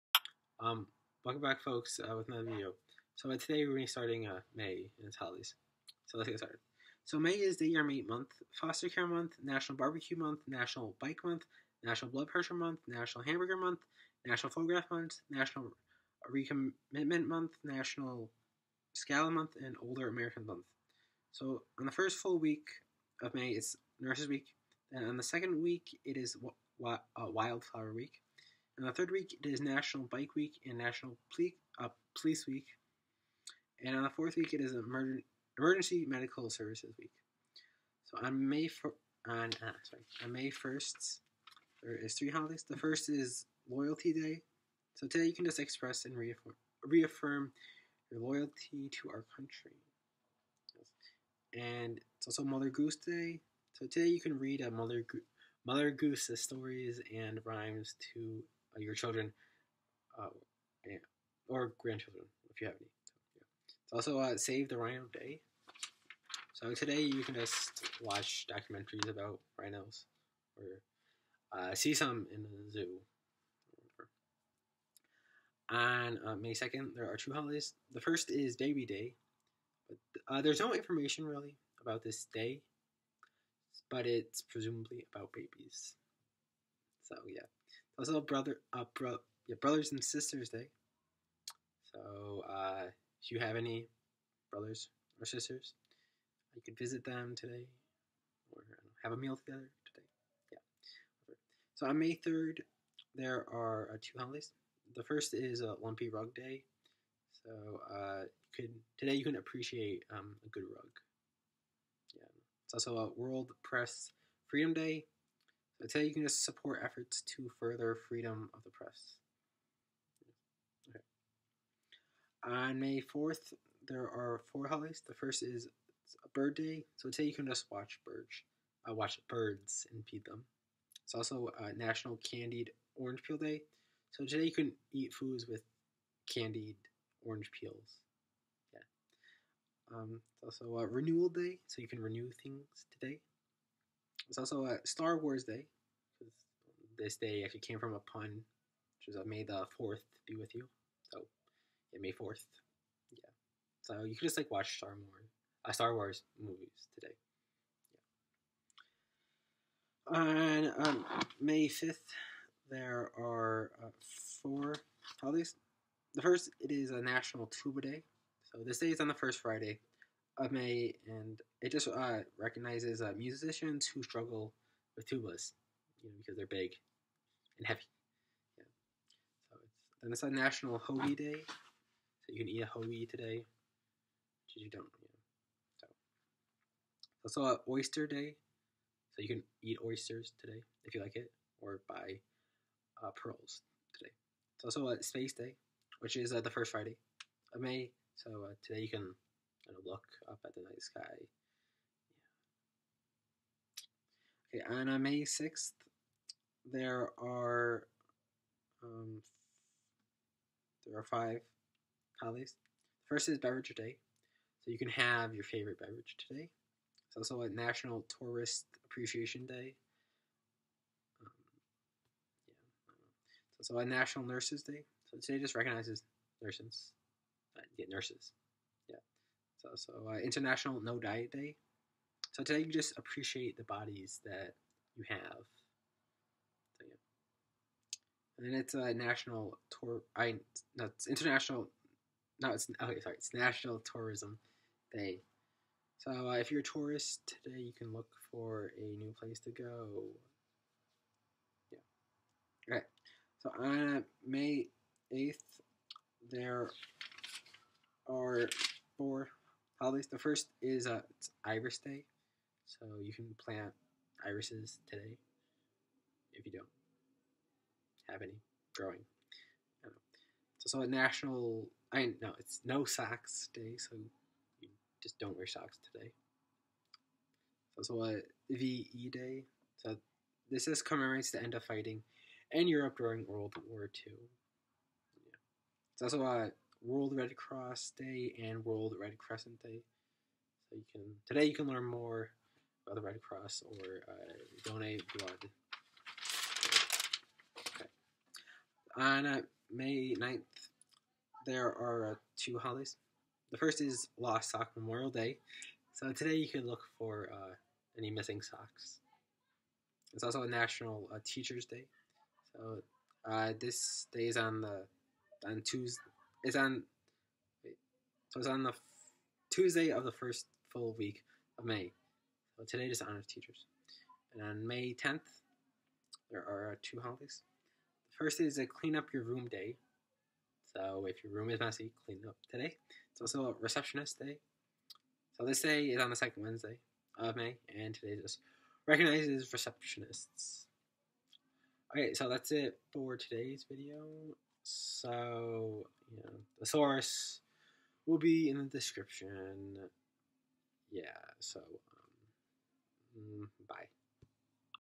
Strange Holidays of May Part 1
If you like to eat hamburgers, make sure to watch this video so that you can be happy for the rest of the year. Source